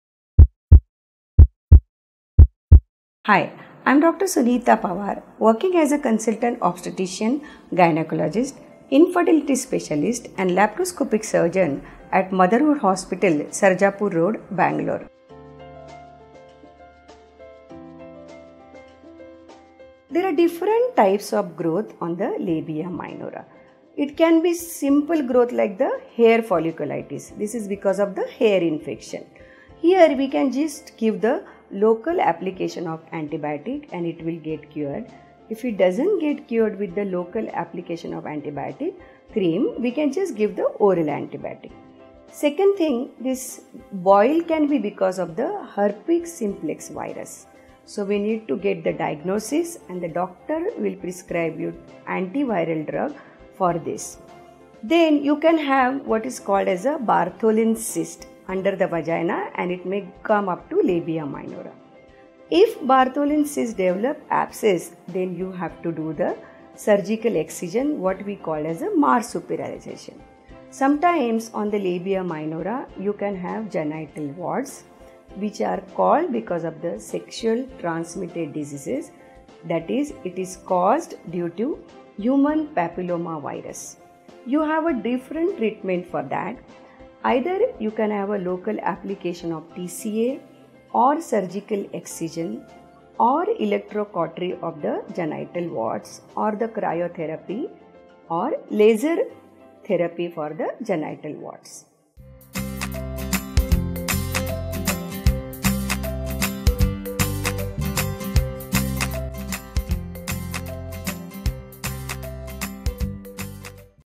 Causes & management of small growth on labia minora - Dr. Sunita Pawar Shekokar
There are different types of growth on the labia minora. It can be simple type of growth like the hair folliculitis. This can be because of the hair infection. Here we can give the local application of antibiotic and it will get cured. If it doesn’t get cured with the local application of antibiotic cream we can give the oral antibiotic. Second thing the boil can be because of the herpes simplex virus. So we need to get the diagnosis and the doctor will prescribe you anti viral drug for this. Then you can have what is called as the Bartholin cyst under the vagina and it may come up to the labia minora. If Bartholin cyst develop abcess then you have to do the surgical excision what we call as the marsupialisation. Sometimes on the labia minora you can have genital warts which are caused by the sexually transmitted diseases i.e. it is cause due to human papilloma virus. You have a different treatment for that. Either you can have a local application of TCA or surgical excision or electrocautery of the genital warts or cryotherapy or laser therapy.